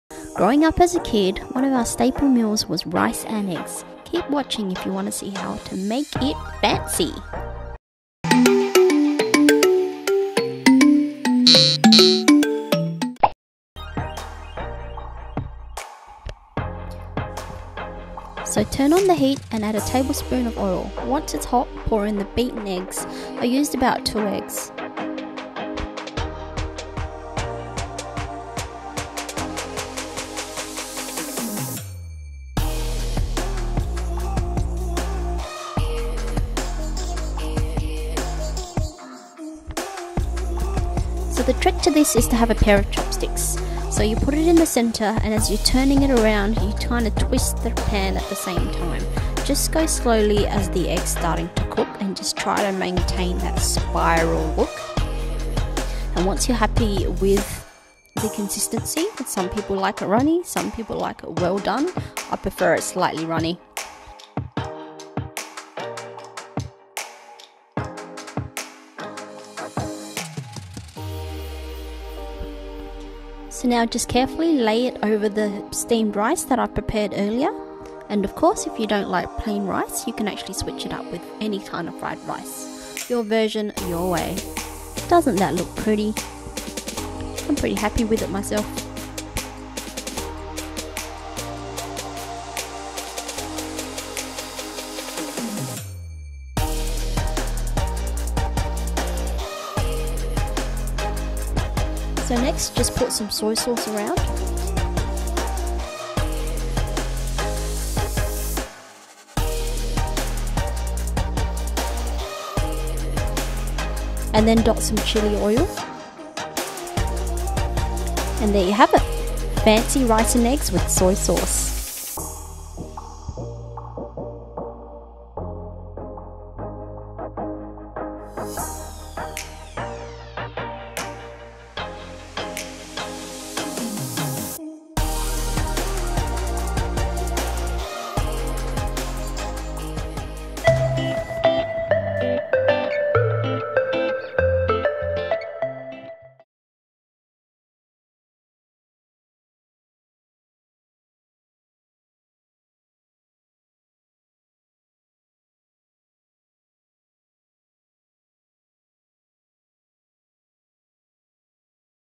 My Fancy Eggs with Rice
This is one of the finest in simple foods growing up in an Asian household. This would be one of my go-to meals when I'm super lazy. This video is more about trying the spiral egg technique and boosting up your meal appeal! believe it or not, this is actually the first time using this technique and wanted to try it out. Check out the youtube video that inspired me to give it a go. with more practice, I should get a more uniform and circular spiral. If I can do it so can you.